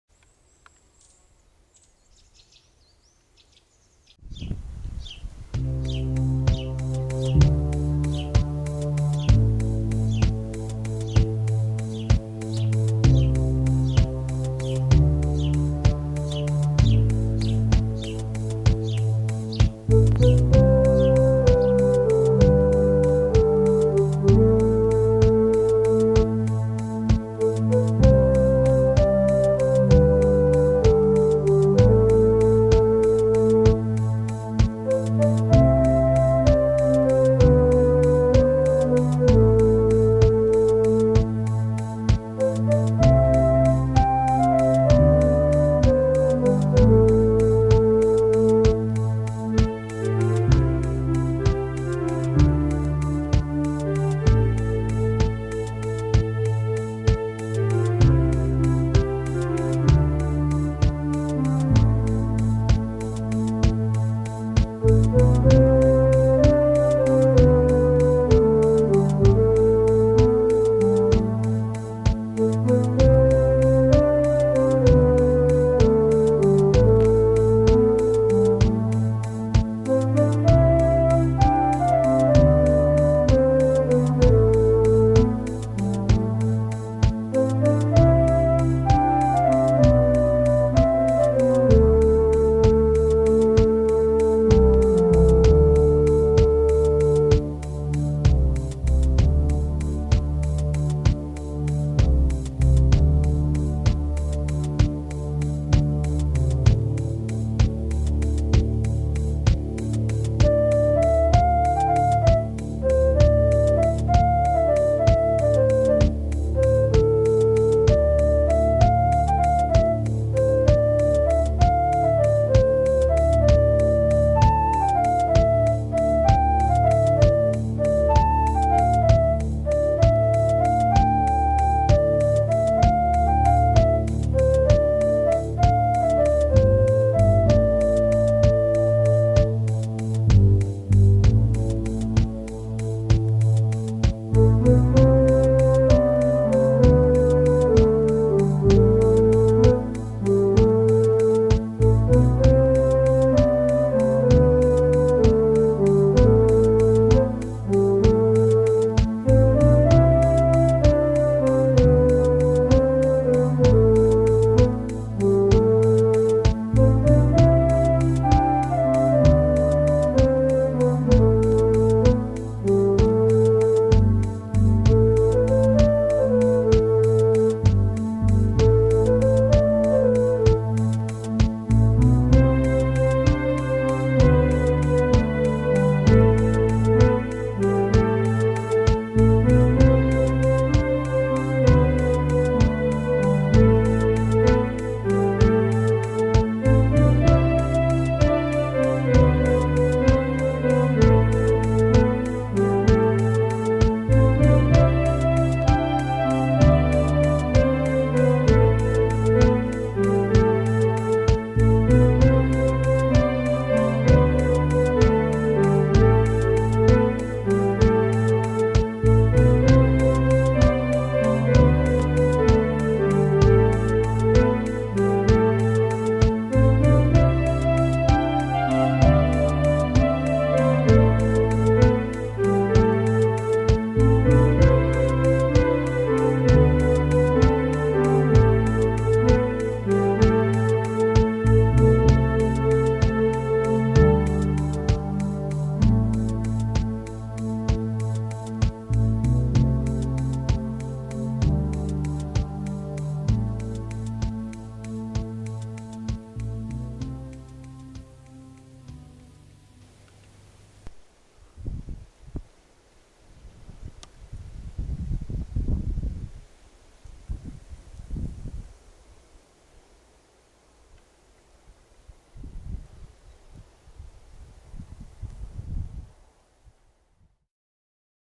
Butterflies of Dorset 2009 Pt 3 Lycaenidae
Part 3 of 5 features, Small Blue, Brown Argus, Holly Blue etc